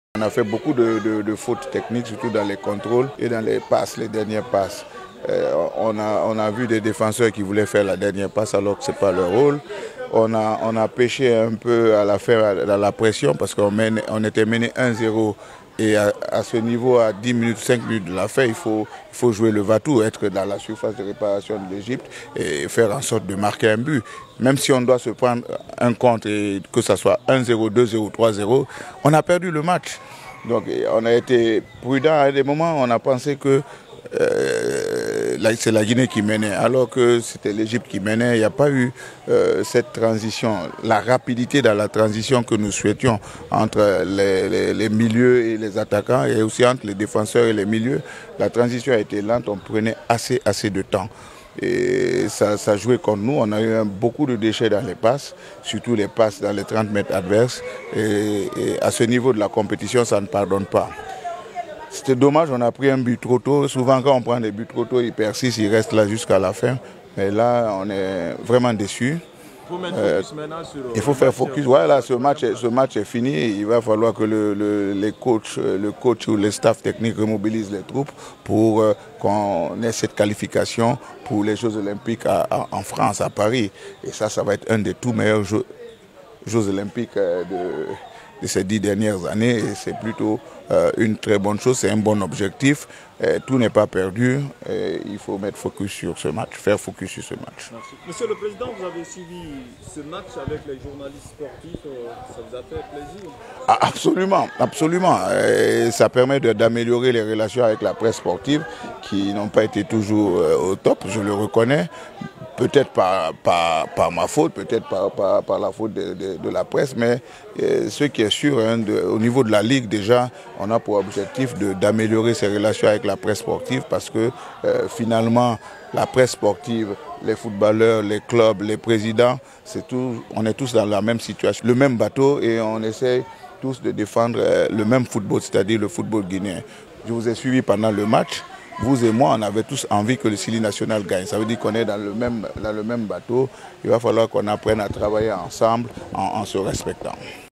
Ancien international et actuel président provisoire de la LGFP, Lucien Guilao a regardé sur...
Ancien international et actuel président provisoire de la LGFP, Lucien Guilao a regardé sur le petit écran le match : Égypte vs Guinée.  Au terme, il a donné son point de vue sur la défaite du Syli espoir